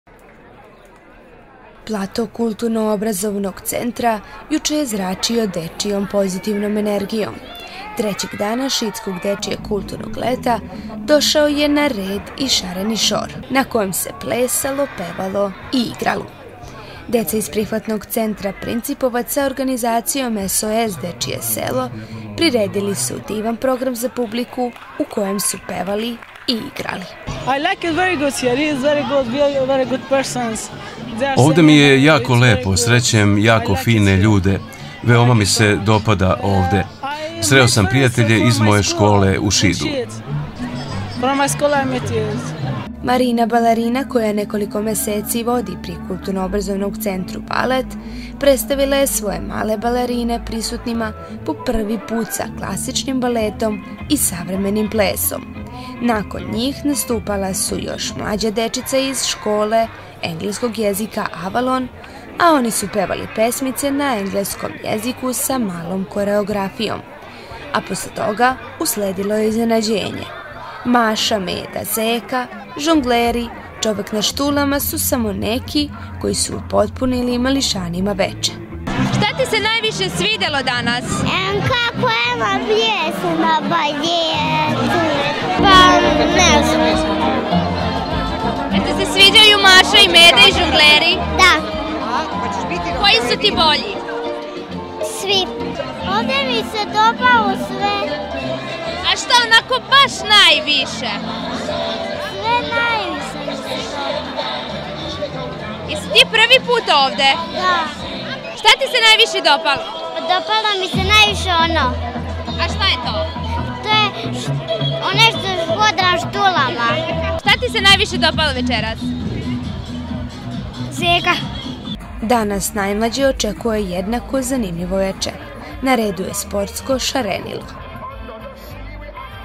Šareni šor - Dečje kulturno leto
Na platou Kulutrno obrazovnog centra u okviru šidskog dečjeg kulutrnog leta, organizovan je šareni šor. Nastupala su deca iz prihvatnog centra Principovac, deca koja pohađaju engleski jezik u školi Avalon i Marina sa svojim balerinama.